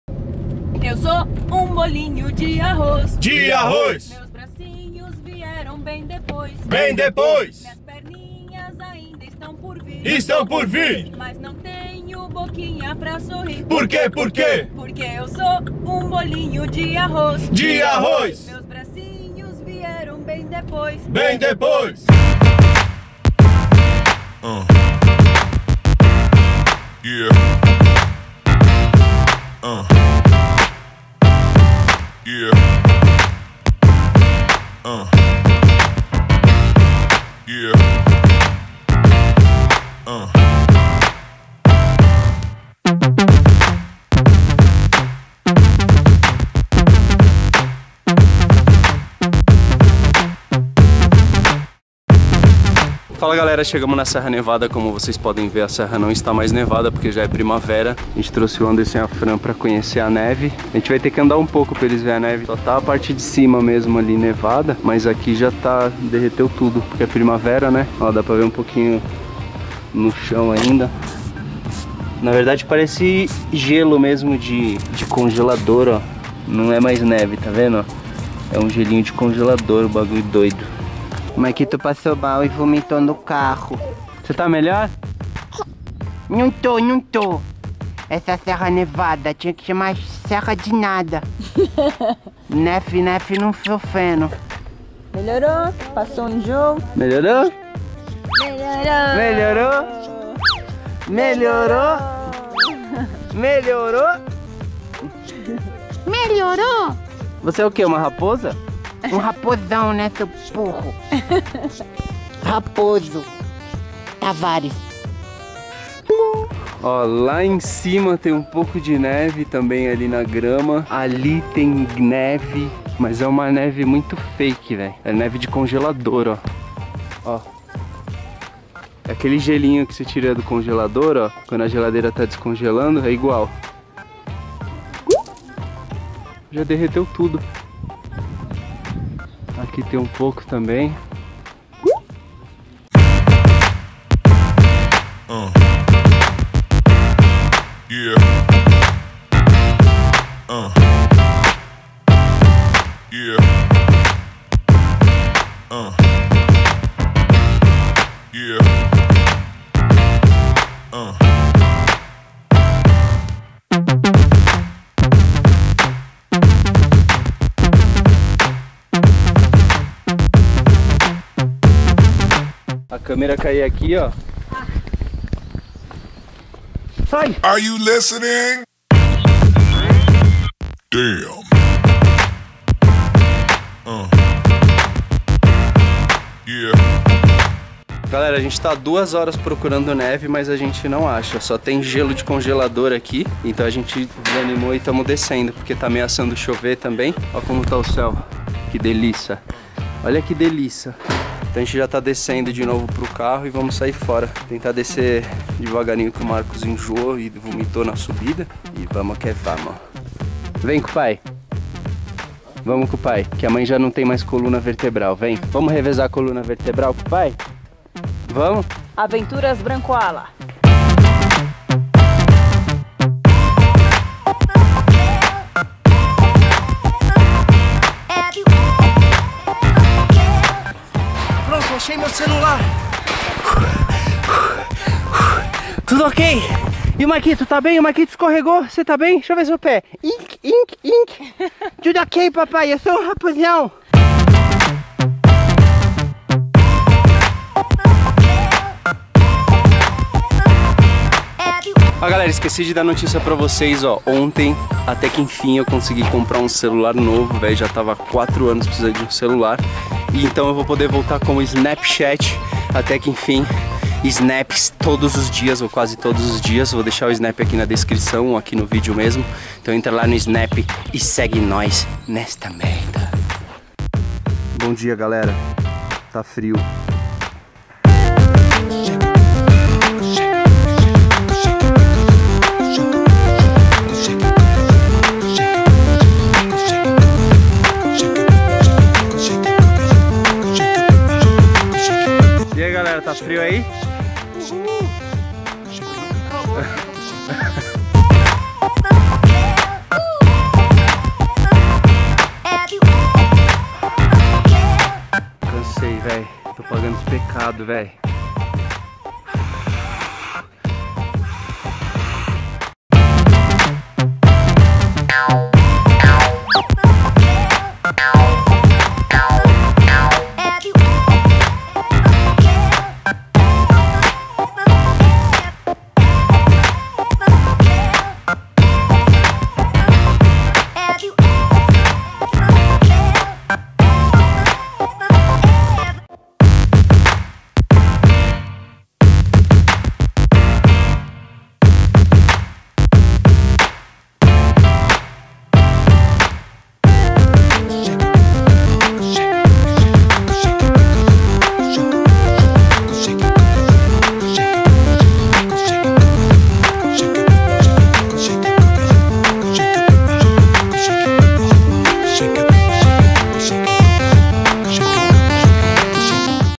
Uma Aventura Congelante em Sierra Nevada Granada Primavera sem Neve Inverno sem Frio [Daily Vlog]
Uma Aventura Congelante em Sierra Nevada Granada Primavera sem Neve Inverno sem Frio na Espanha [Frozen Daily Vlog]